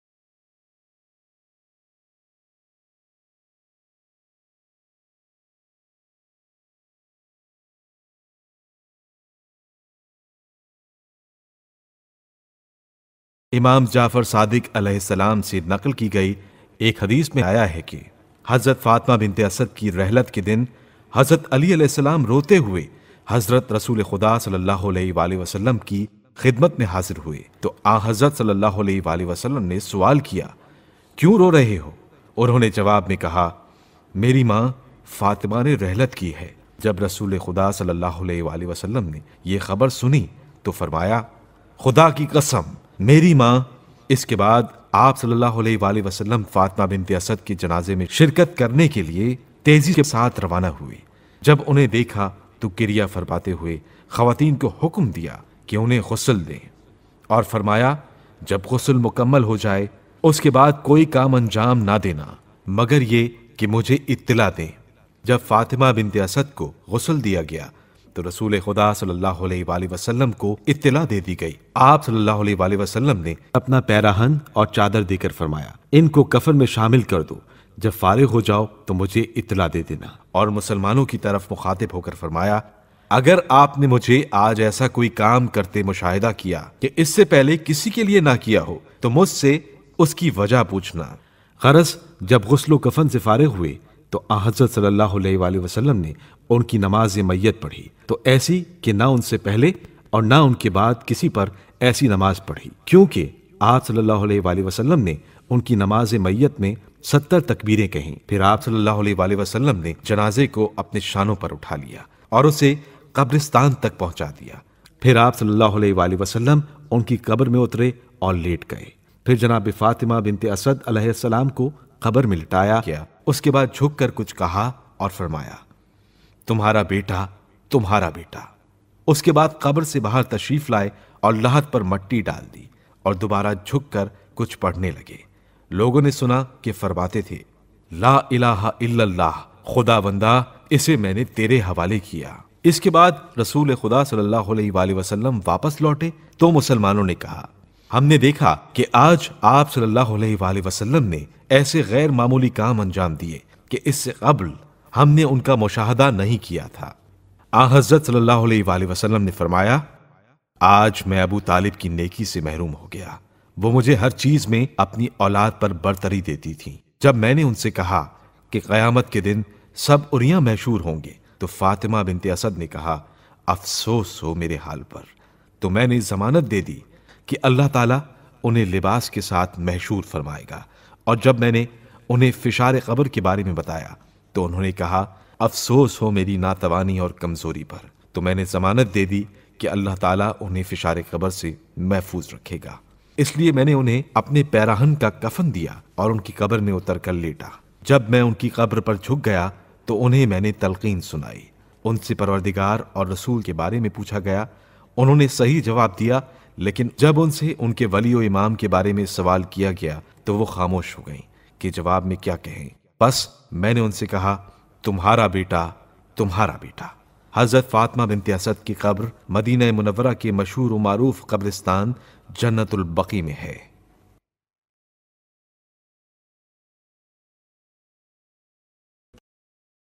Sawanih Hazrat Ali (A.S.) | Episode 04
قسط 04
عصمت کے درخشاں ستارے
سوانح حیات حضرت علی علیہ السلام
حضرت علیؑ کے والدین
مؤلف: محمد رضا مرچنٹ
اردو آواز: محسن شہید مرزا
عربی آواز: مولانا رضا علی عابدی